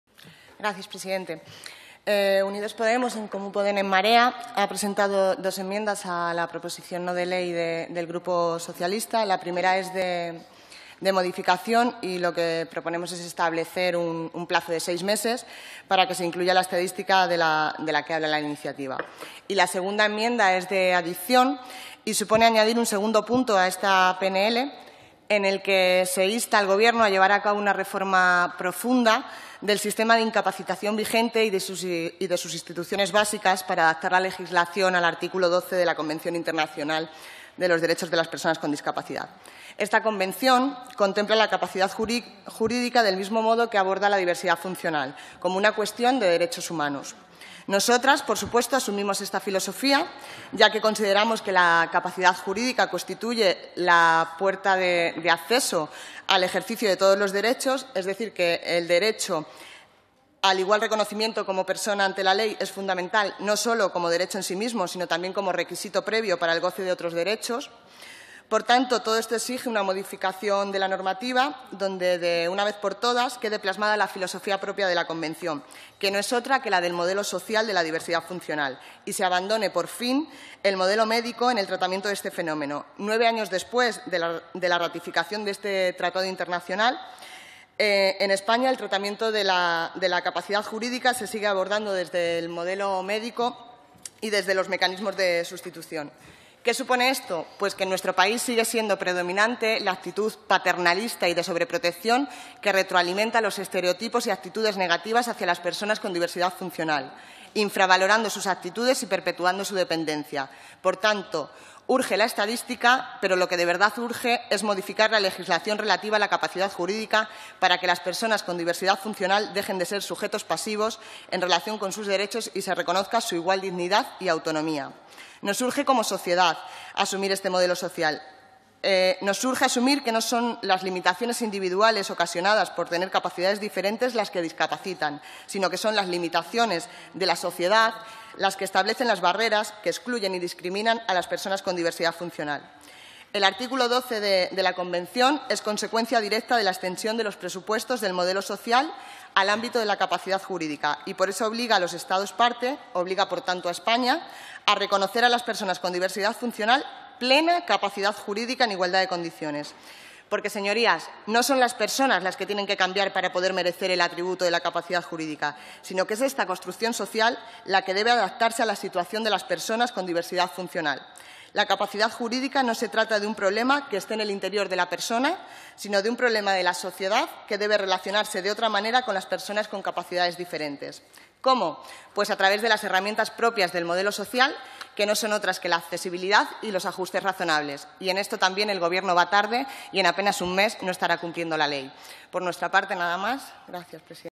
Teresa Arévalo en la Comisión para las Políticas Integrales de la Discapacidad el 15 de Noviembre
Sobre la proposición no de ley relativa a la confección de una estadística pública sobre procedimientos judiciales de incapacitación o de modificación de la capacidad de obrar.